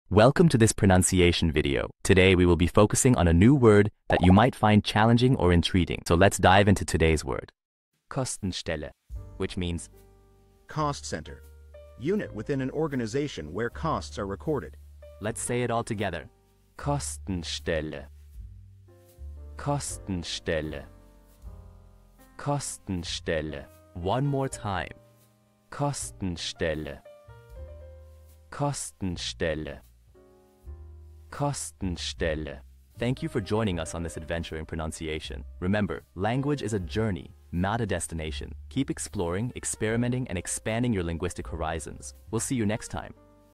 How to pronounce Kostenstelle
Master the Pronunciation of 'Kostenstelle which Means cost center' - which means : Cost Center: Unit within an organization where Costs are Recorded. 📝 with @PronunciationGerman 🎓🗣️ - [Your Guide to German]

Learn to pronounce 'Kostenstelle' perfectly in German with our easy and engaging tutorial by @PronunciationGerman! 📖📽️ In this comprehensive video, we provide you with native speaker examples 🗣️👂 and phonetic breakdowns 🎵 to ensure you get the pronunciation just right.

How to say Cost centre in German